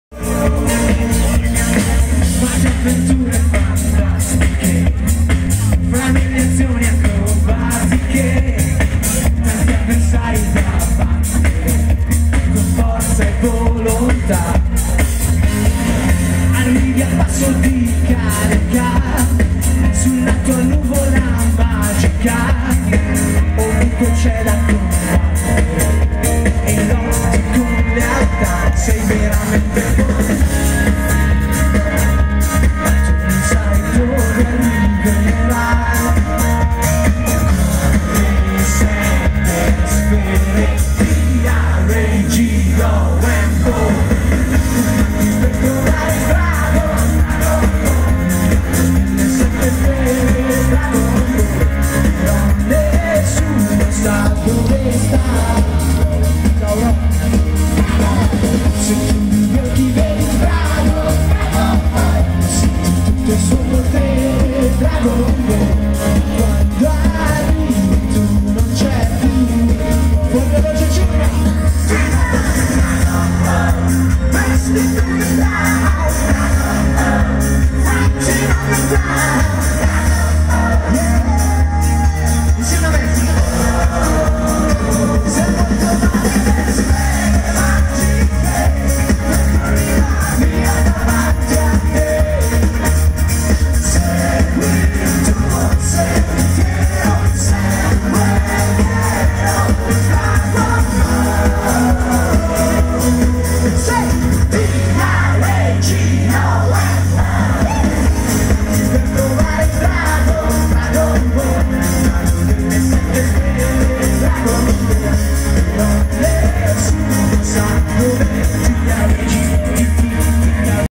Giorgio Vanni e I Figli di Goku + Dj FeLLeAs @ Sagra dei Fumetti Villafranca di Verona (1/3)
"Giorgio Vanni E I Figli di Goku" + Special Event "DJ SET by MAX LONGHI Dj FeLLeAs" alla Terza Edizione de "La sagra dei fumetti", Castello Scaligero di Villafranca di Verona.